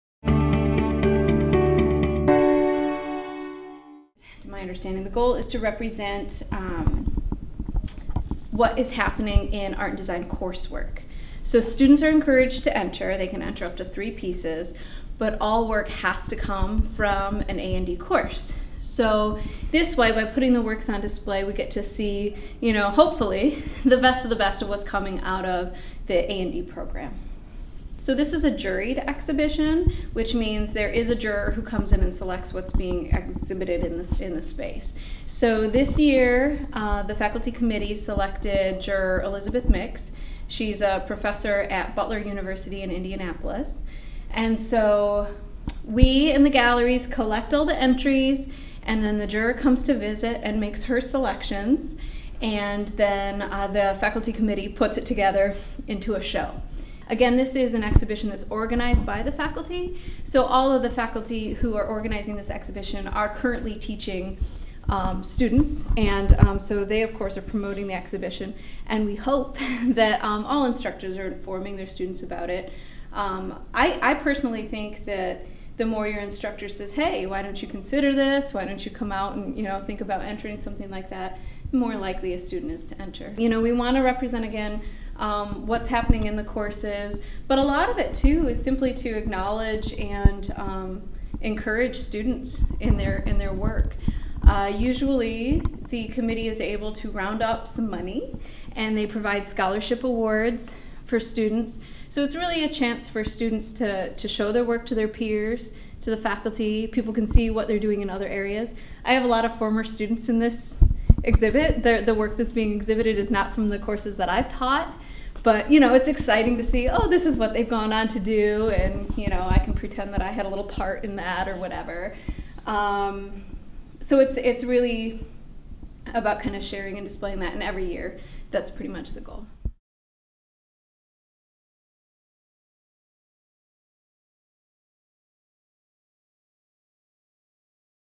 The Purdue Exponent - Undergarduate Art Expo
Liz Erlewine, Coordinator of the Rueff Art Gallery, gives some insight involving the current displays at the Patti and Rusty Rueff Art Gallery located in Pao Hall, at Purdue University